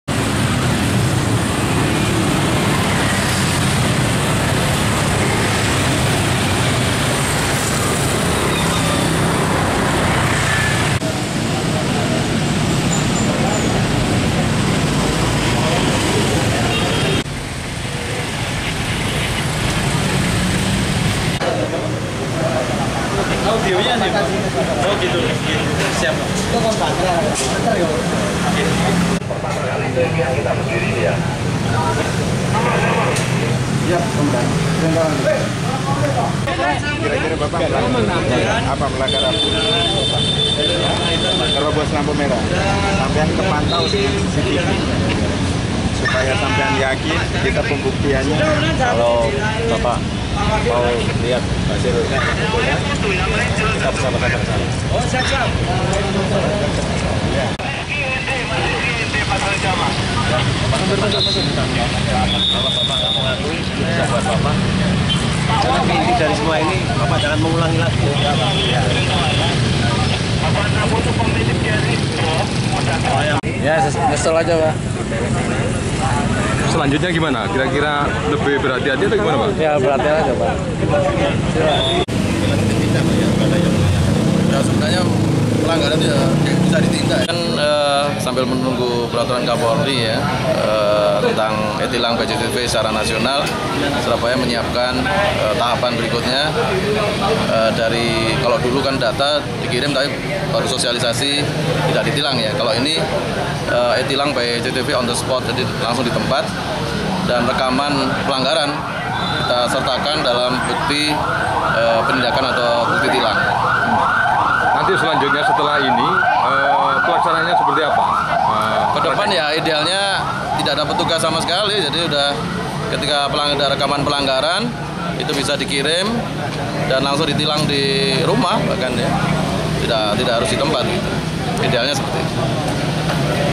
Dishub Mulai Uji Coba E-Tilang Bagi Pelanggar Lalu Lintas, Pelanggaran Terpantau Lewat CCTV
Melalui CCTV yang dipasang di traffic light persimpangan jalan atau zebra cross serta tempat strategis lainnya, semua pelanggaran lalu lintas bisa terekam kamera.

Semua pelanggaran lalu lintas berupa marga,rambu dan lampu TL ini akan diproses sesuai UU yang berlaku. Ketahuan melanggar akan langsung ditilang.